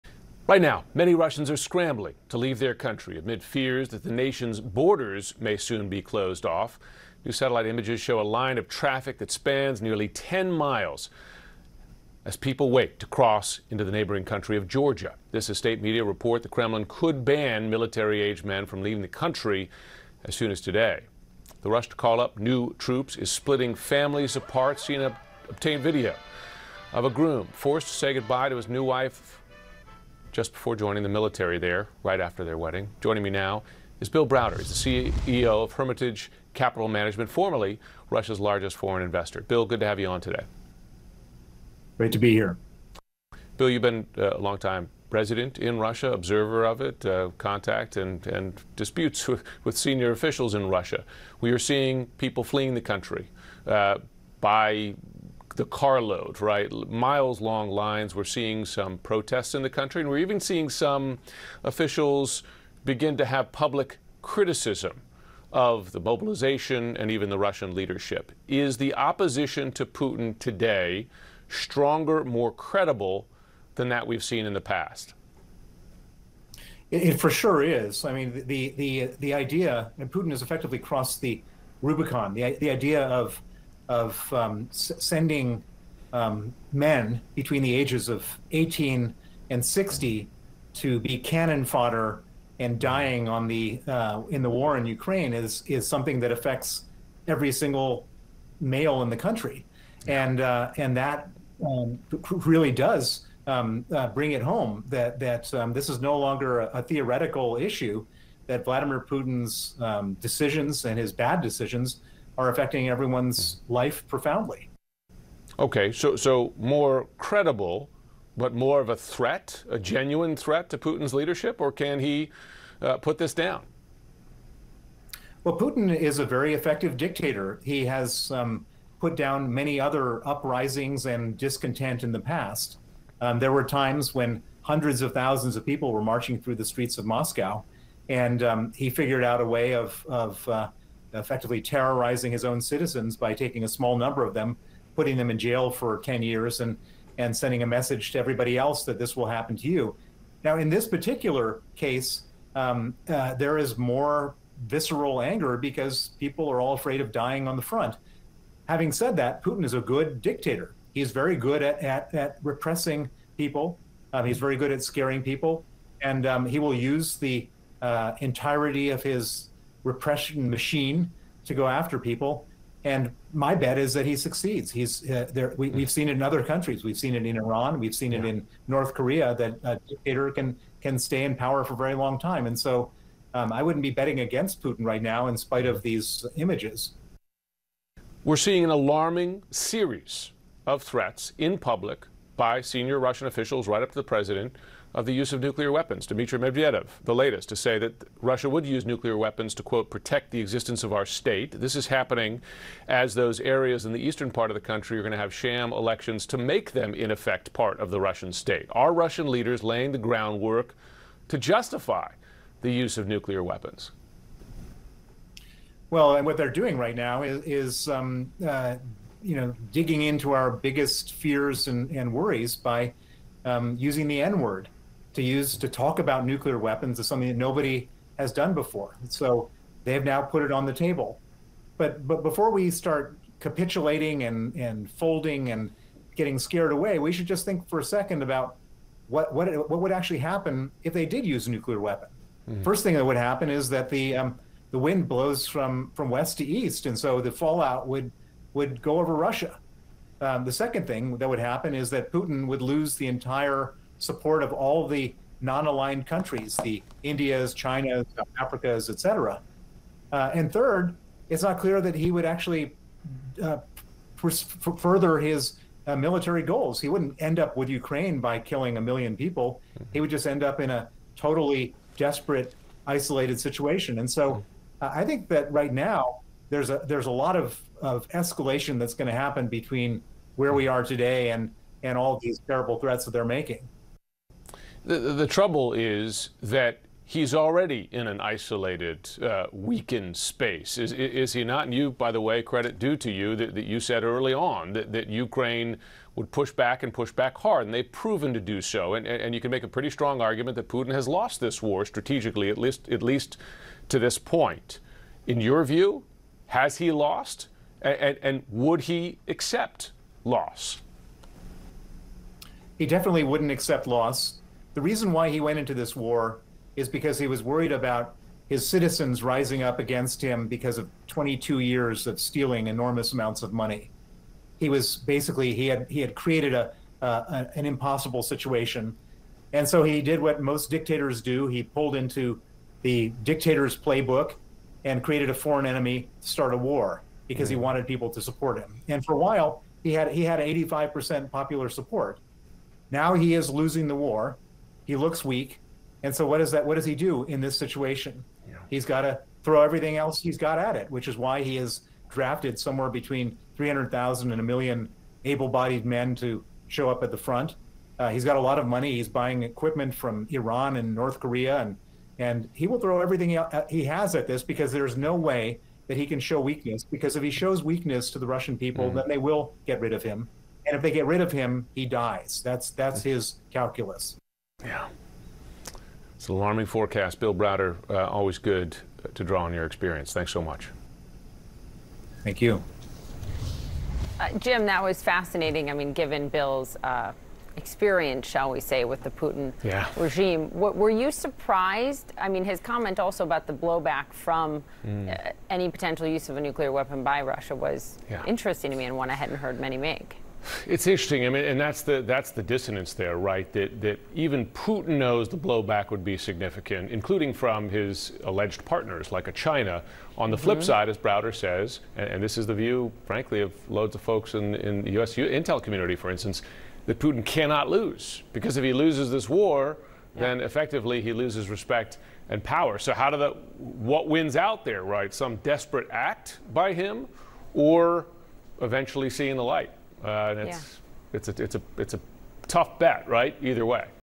'Putin has effectively crossed the Rubicon': Putin critic Bill Browder on backlash in Russia
Hermitage Capital Management CEO Bill Browder and CNN's Jim Sciutto discuss the "partial mobilization" order by Russian President Vladimir Putin.